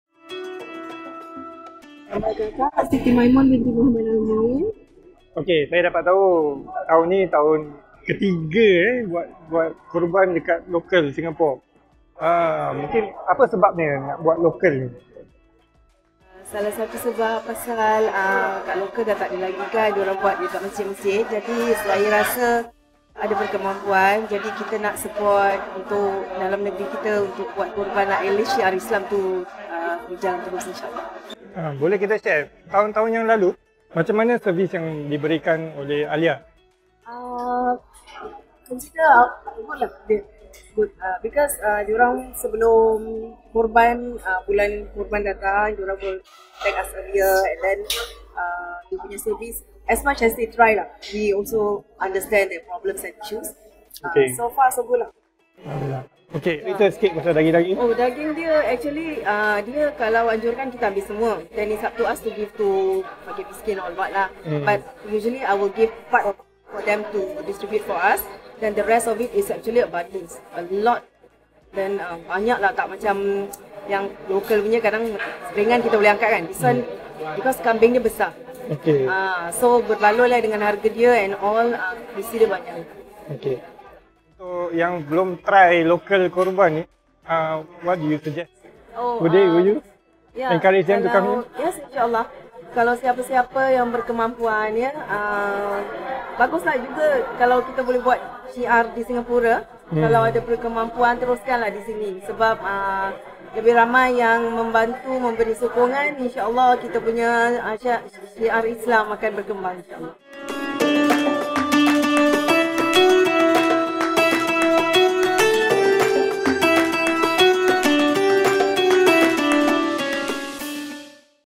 2023 Qurban Interview 1
This is the 3rd year, Puan Maimon is doing the local Qurban with Aliyah's Pte Ltd.

The Qurban team will be at Hay Dairies farm this weekend from 10am to 3pm.

Bring along your family to have the experience to feed the goats and buy fresh milk from the farm.